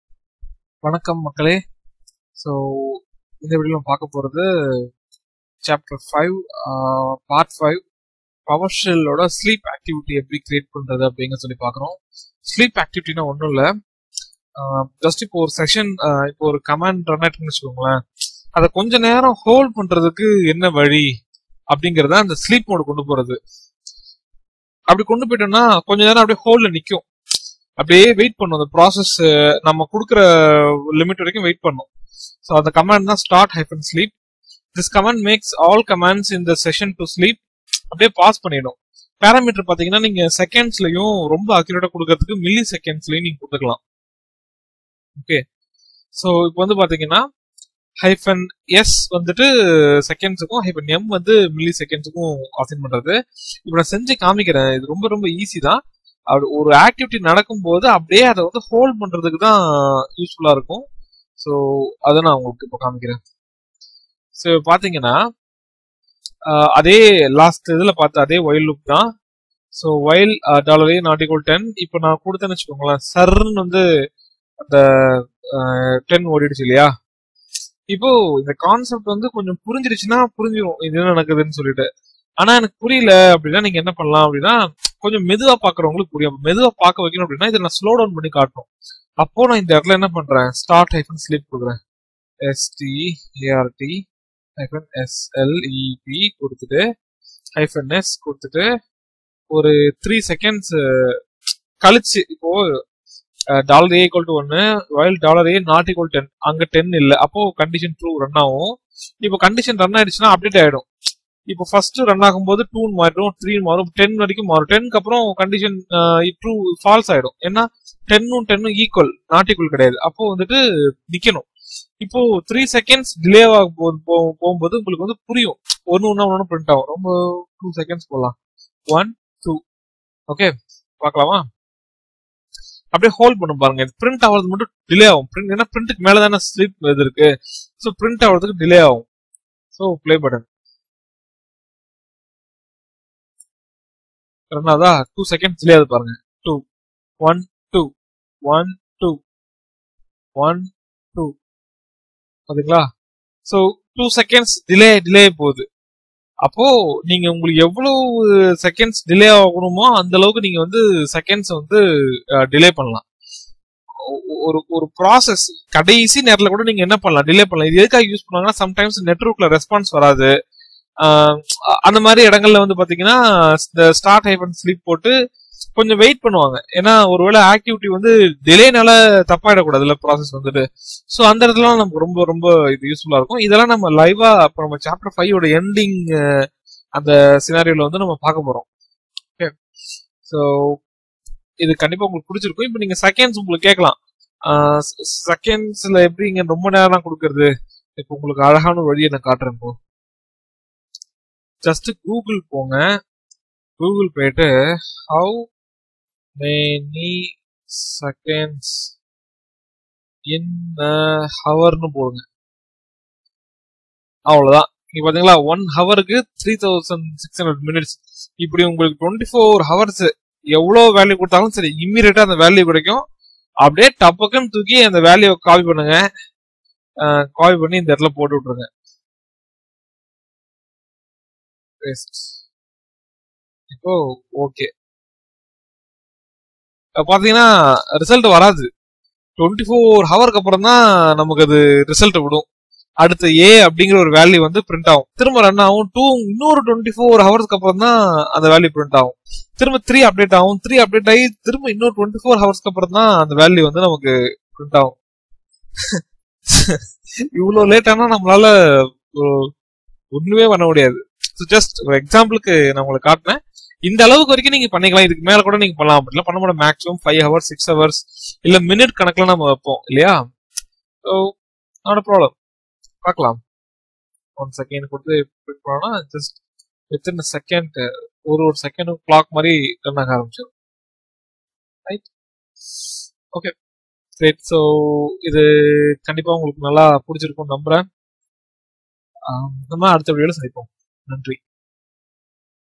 Powershell in tamil - Chapter 5 - Sleep or hold activity - Devops : Powershell training in chennai
In this video you can learn to hold or pause a powershell script for some time.